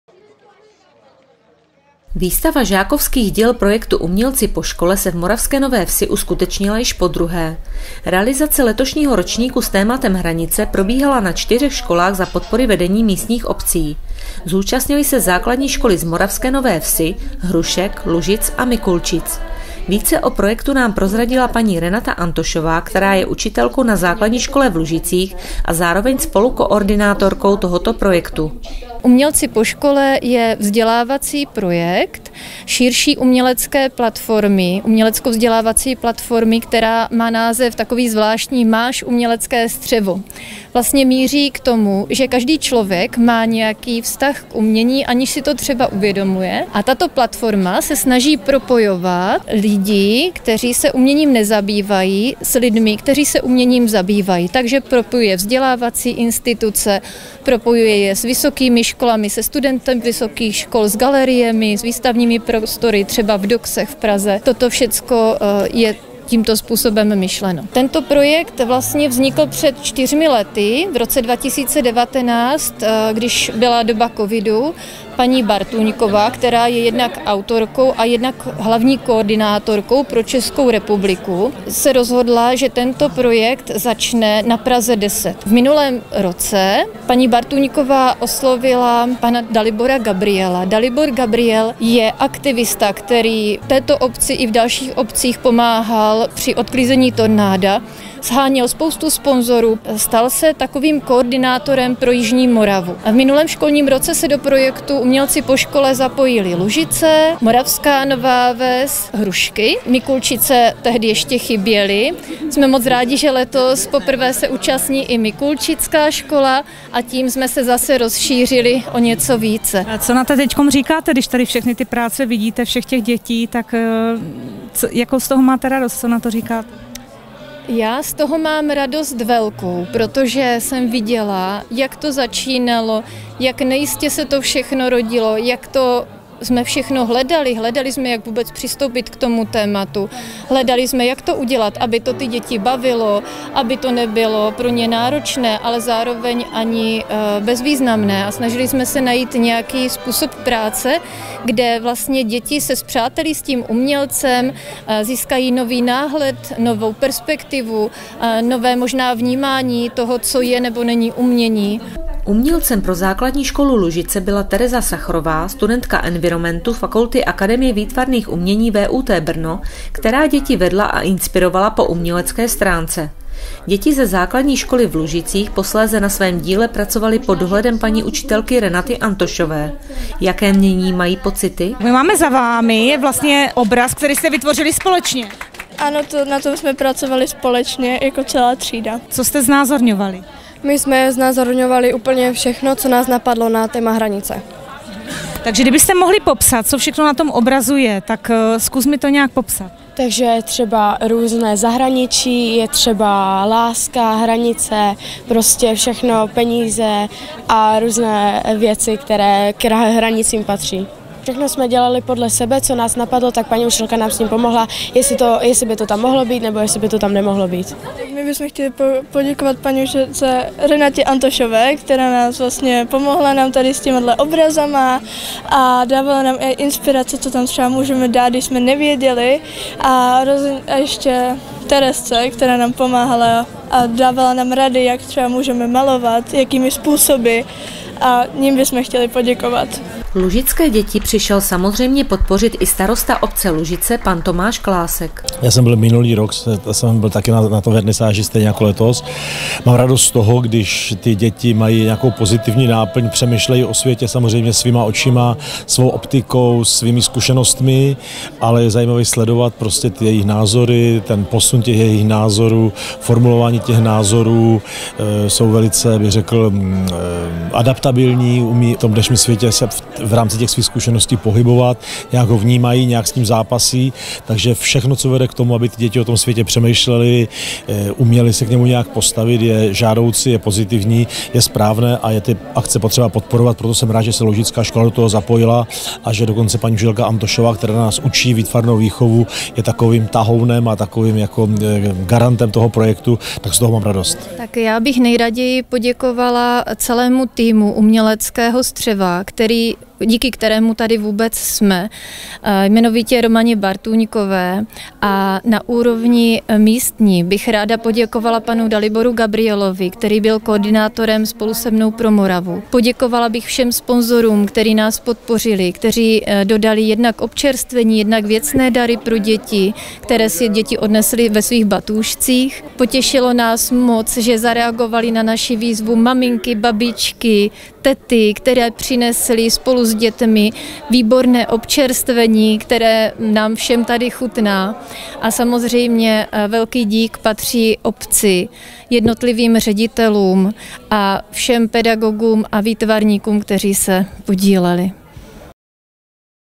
HRANICE byly tématem letošní výstavy a vernisáže projektu Umělci po škole
Výstava žákovských děl projektu Umělci po škole se v Moravské Nové Vsi uskutečnila již podruhé. Realizace letošního ročníku s tématem „HRANICE“ probíhala na čtyřech školách za podpory vedení místních obcí. Zúčastnily se základní školy z Moravské Nové Vsi, Hrušek, Lužic a Mikulčic. Více o projektu nám prozradila paní Renata Antošová, která je učitelkou na ZŠ v Lužicích a zároveň spolukoordinátorkou tohoto projektu.